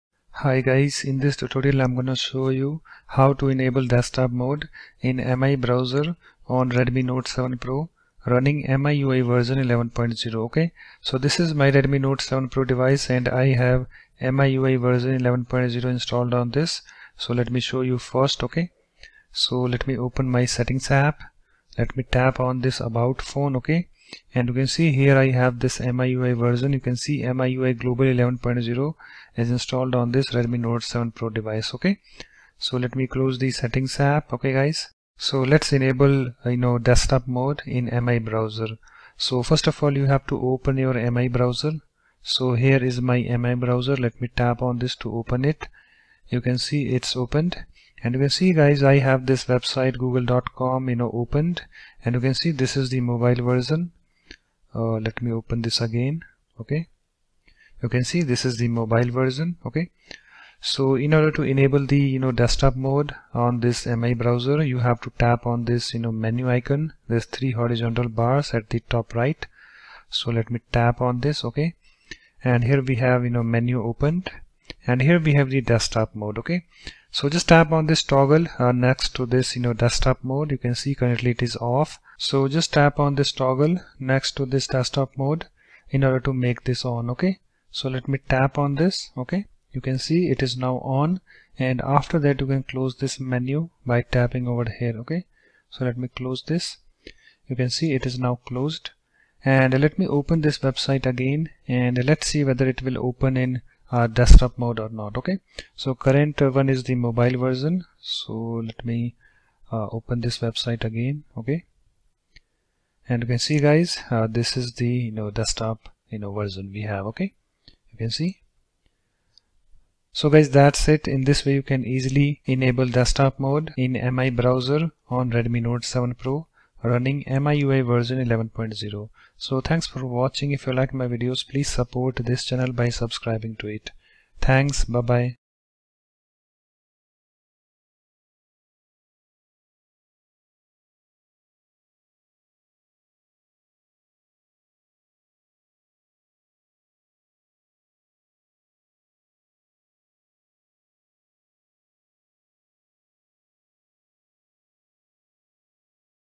How to Enable Desktop Mode in Mi Browser on Redmi Note 7 Pro(MIUI 11.0)?
Step 1: Open 'Mi Browser' App.

Step 2: Tap on 'Three Bars' at the top right.

Step 3: Tap on 'Toggle' next to 'Desktop Mode' in order to make it 'On.'

It's done.